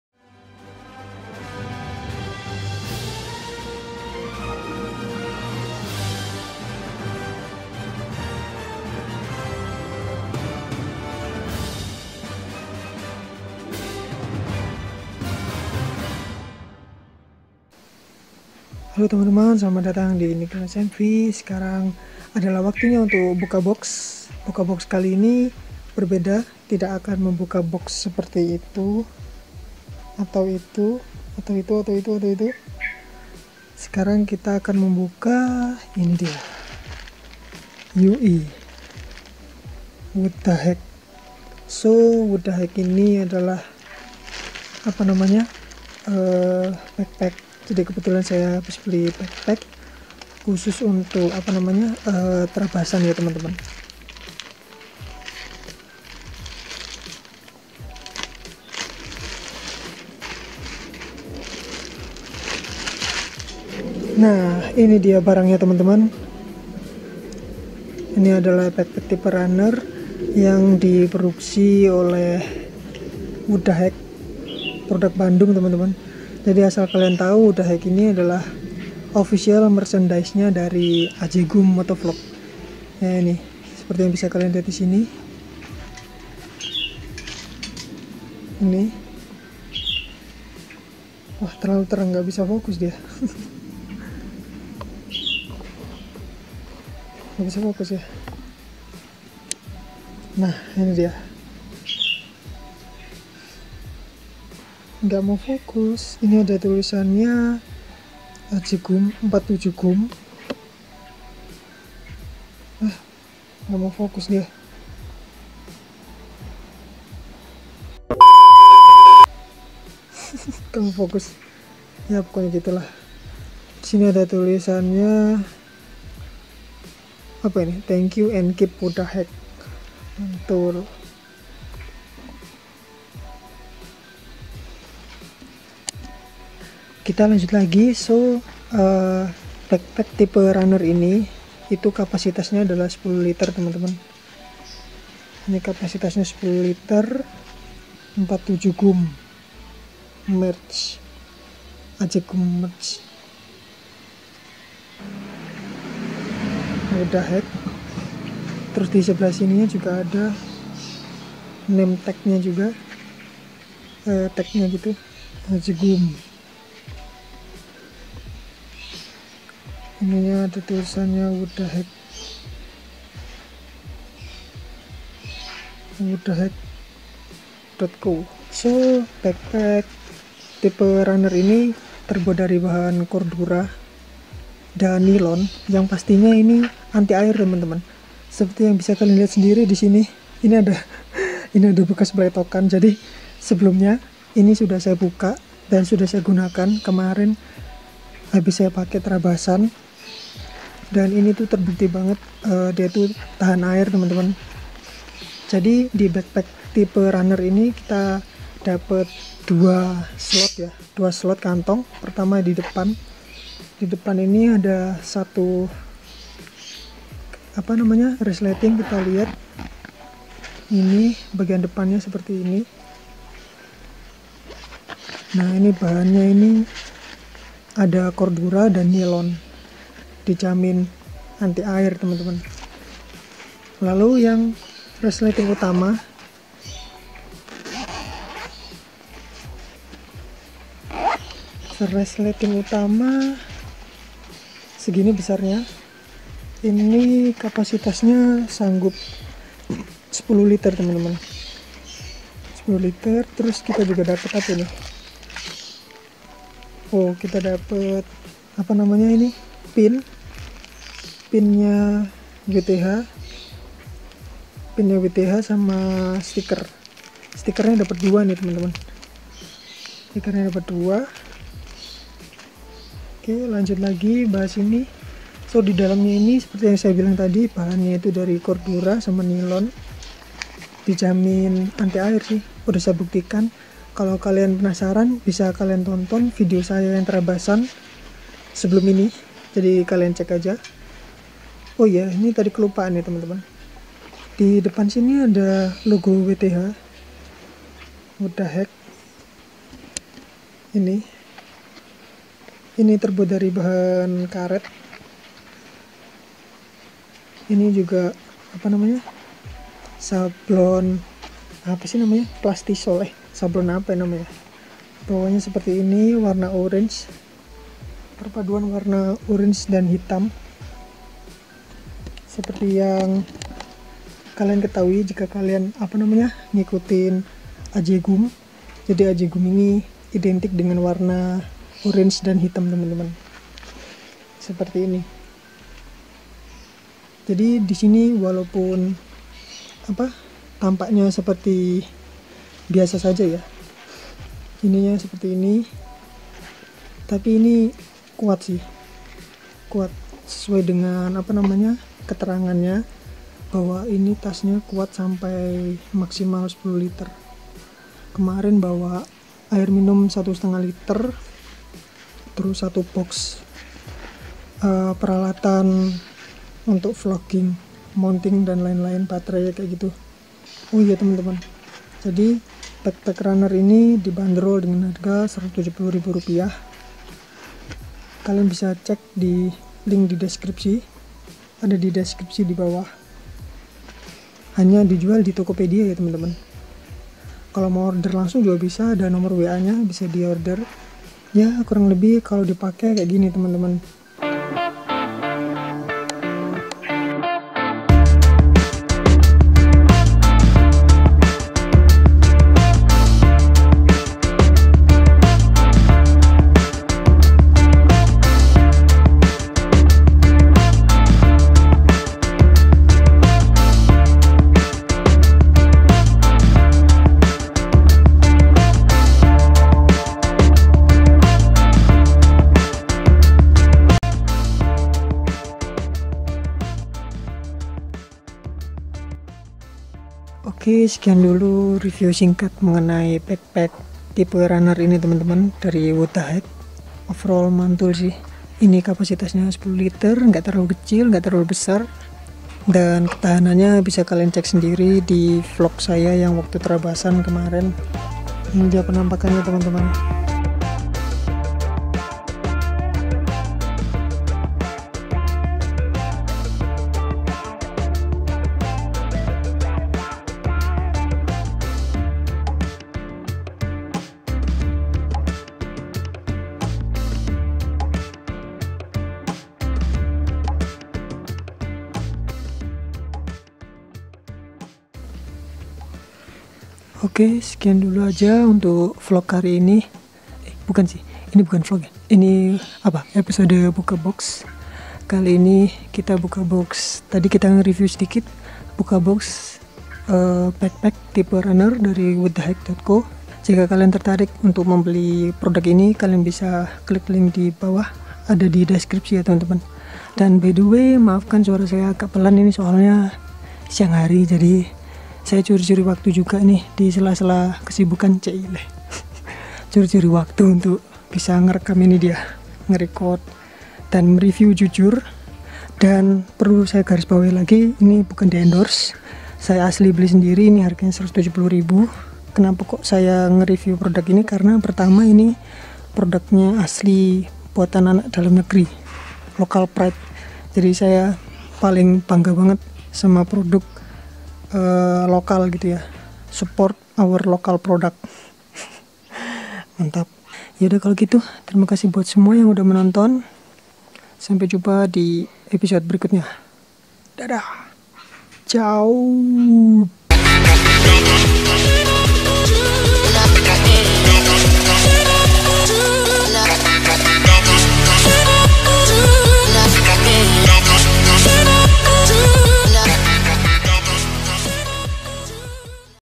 REVIEW BACKPACK ANTI AIR + MURAH | WUTDAHECK RUNNER 10L
Selamat Datang di BUKABOX Episode 15,

Tas ini cocok banget buat kalian yang hobi trekking, sepedaan, maupun trabasan..ukurannya 10liter, Anti Air dan yang pastinya modelnya cakep dengan perpaduan warna orange dan hitam khas Ajgum...Btw tas ini saya pake waktu trabasan kemaren temen2, videonya bisa kalian cek sendiri yaa..

Saran dan Kritik monggo langsung di kolom komentar yaa :)